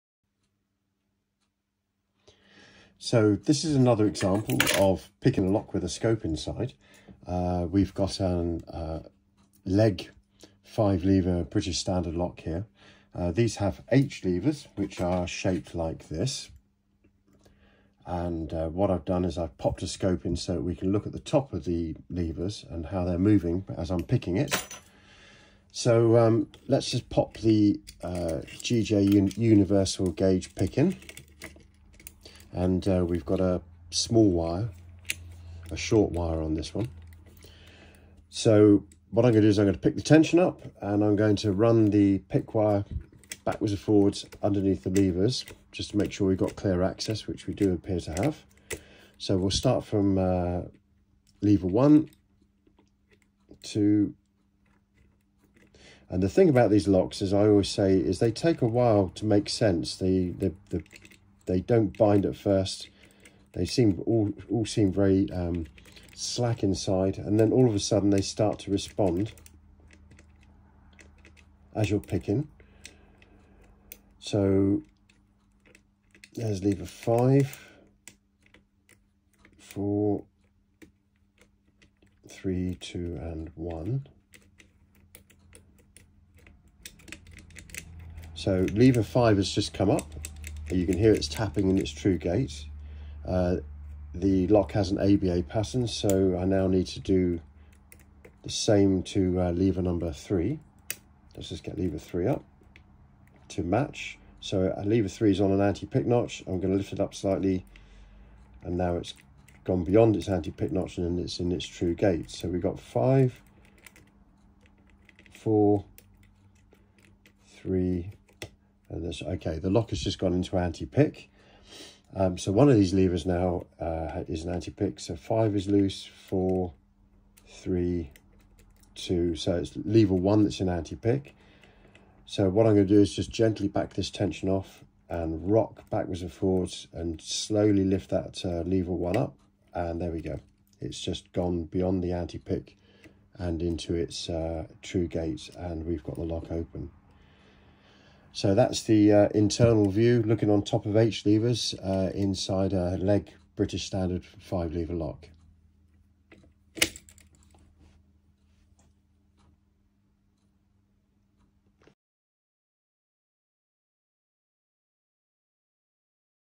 Lock Picking Shorts - Legge 5 Lever picked open with a scope view of the levers
Legge BS 5 Leverage Sash Mortice Lock being opened with a scope showing the internal view.